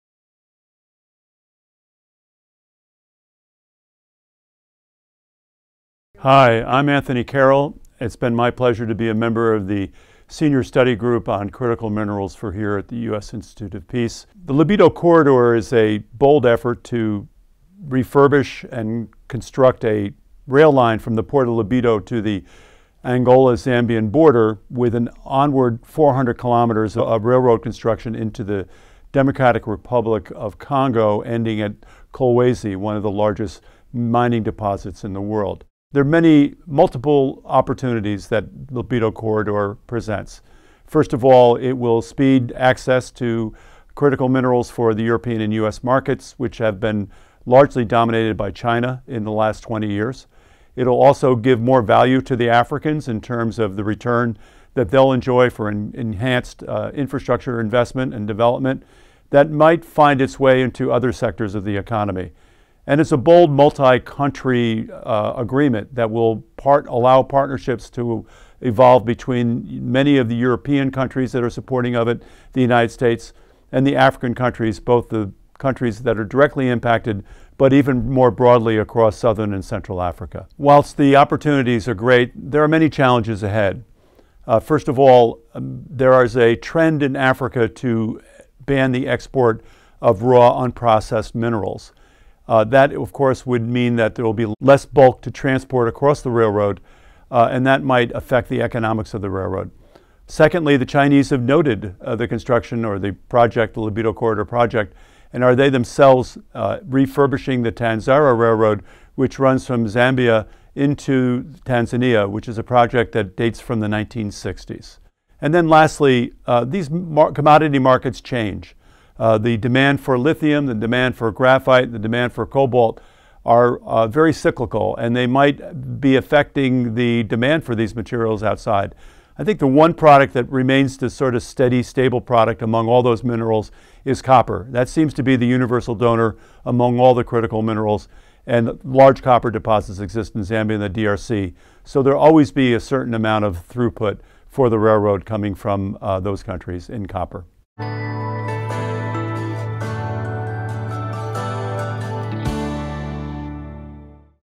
What Is Africa's Lobito Corridor?
The Lobito Corridor is a railway project stretching from the Angolan port of Lobito on Africa’s Atlantic coast to the city of Kolwezi in the Democratic Republic of the Congo, which contains one of the largest mining deposits in the world. Anthony Carroll, a member of USIP's senior study group on critical minerals in Africa, discusses how this multi-country project can help speed access to critical minerals for U.S. and European markets, bolster African economic development and reduce reliance on China for critical mineral supply lines.

00:00 What Is Africa's Lobito Corridor?
00:15 Where is the Lobito Corridor located?
00:36 What are the opportunities?
01:21 What are the challenges?